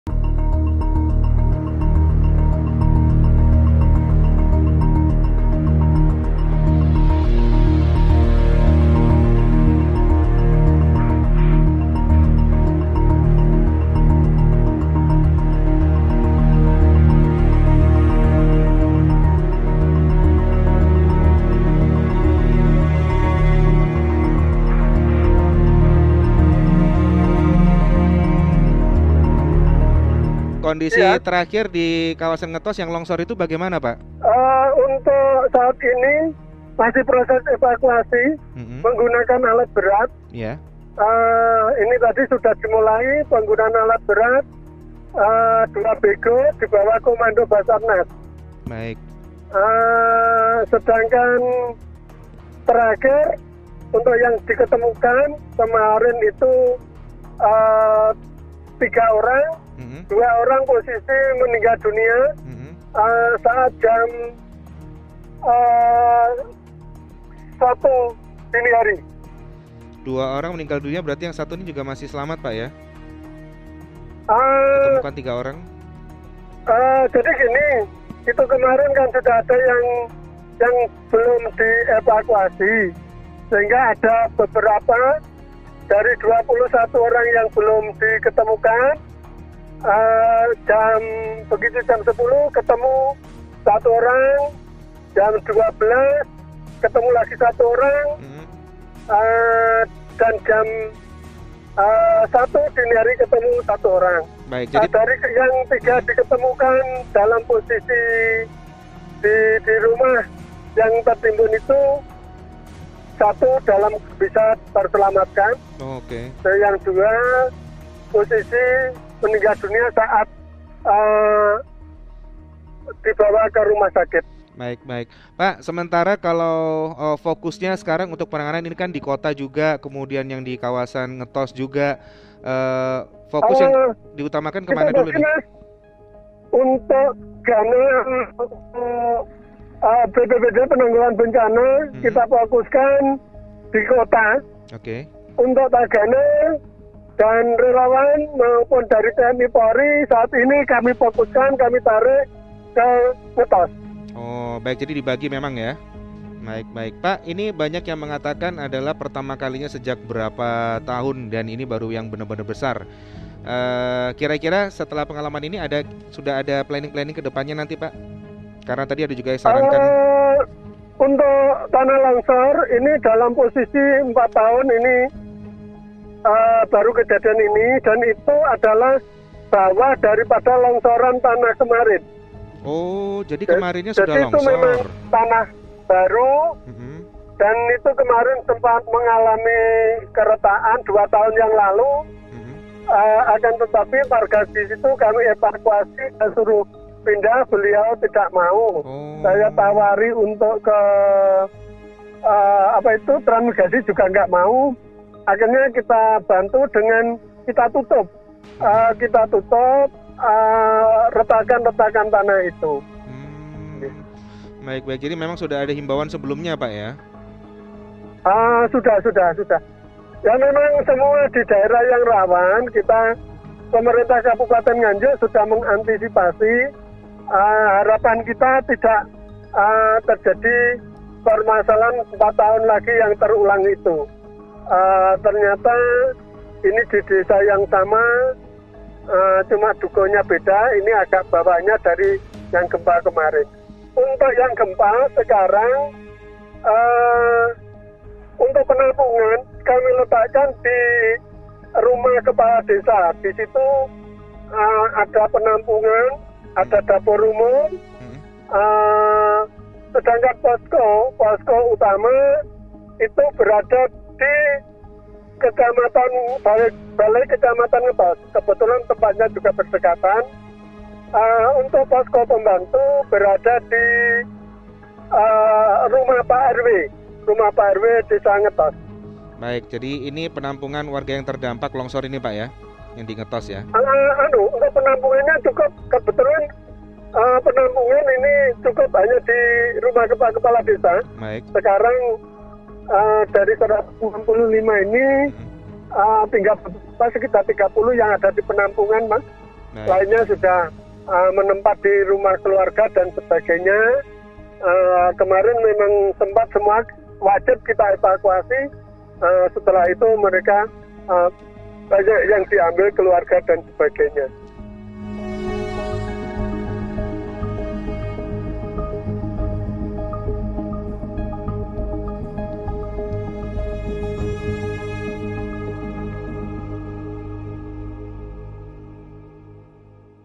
Tanah di Lokasi Longsor Nganjuk Sudah Retak Dua Tahun Lalu
Evakuasi korban bencana alam longsor di Ngetos Nganjuk oleh Tim Gabungan masih terus dilakukan, Senin (15/2/2021) siang. Ada 16 orang yang masih dalam proses pencarian dan korban selamat lainnya sudah ditempatkan ke penampungan-penampungan yang disediakan.

Tim evakuasi juga dibagi ke lokasi banjir di tiga kecamatan tengah kota.